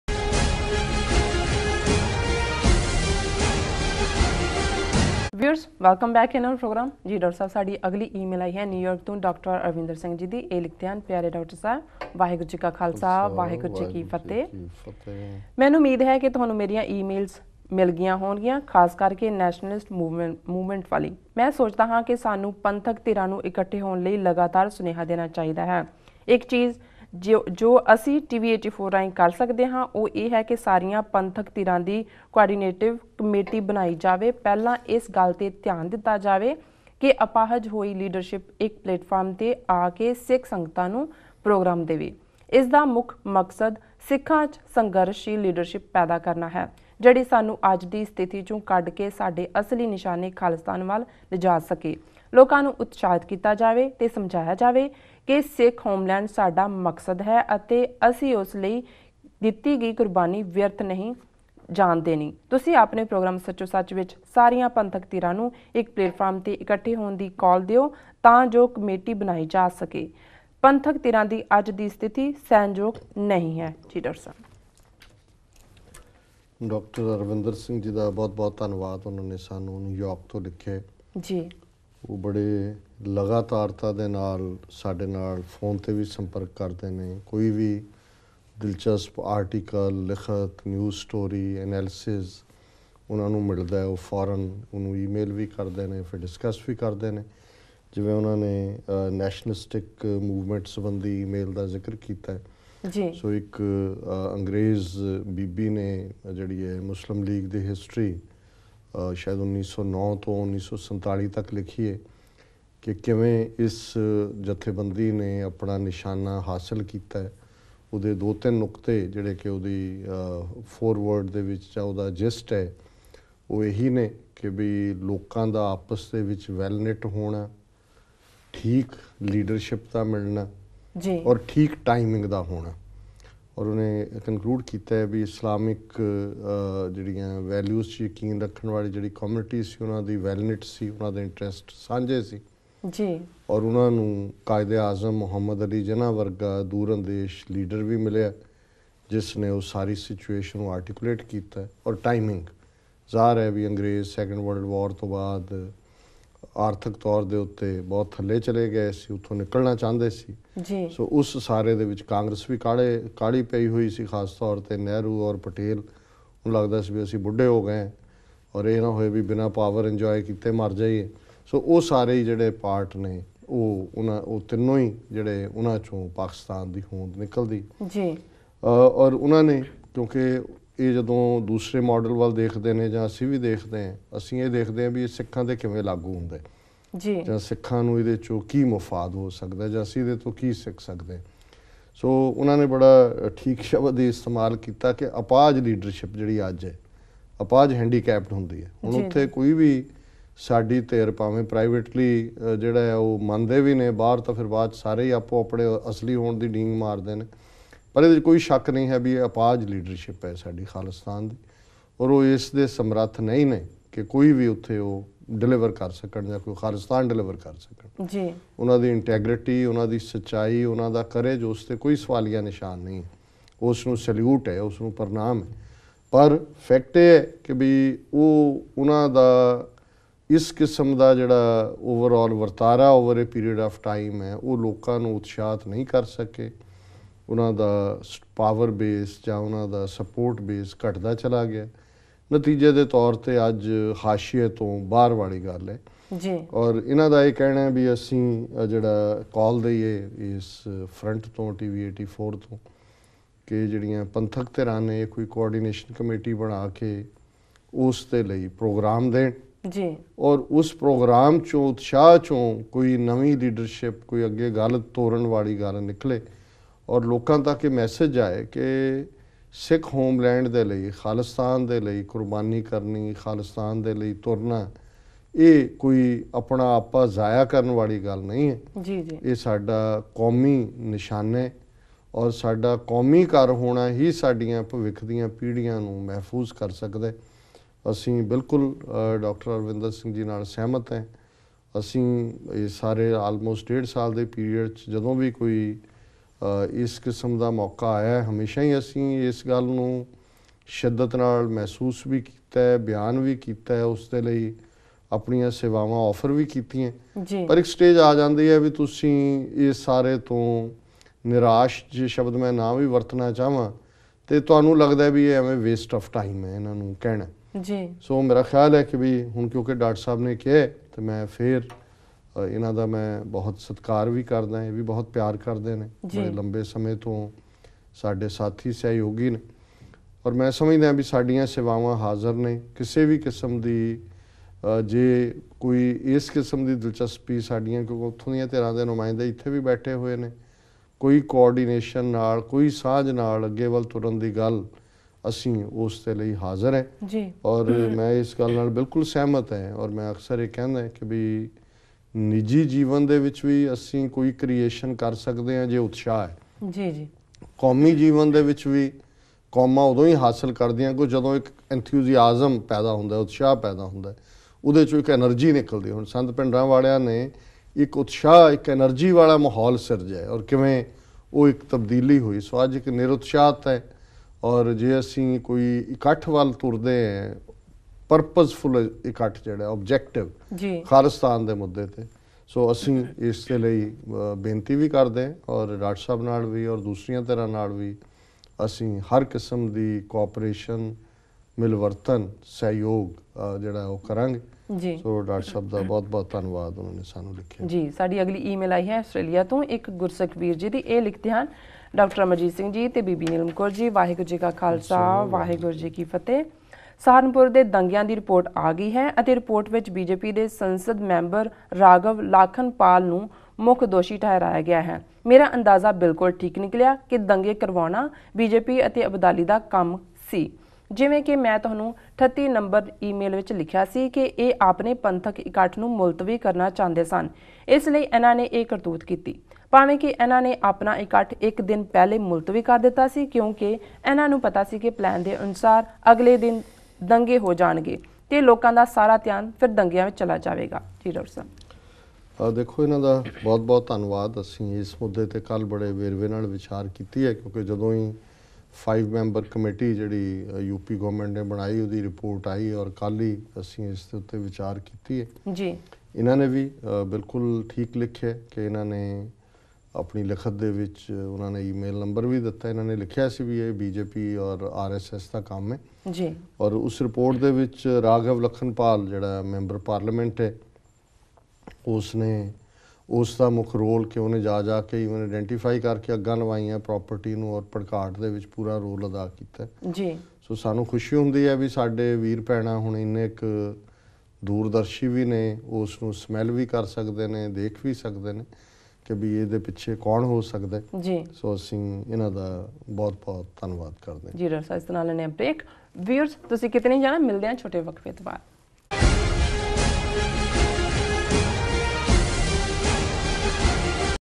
SOS 8/19/14 Part.3 Dr. Amarjit Singh on : 'Handicapped' Khalistani Leadership Should Unite To Lead
Dr. Amarjit Singh on : 'Handicapped' Khalistani Leadership Should Unite To Lead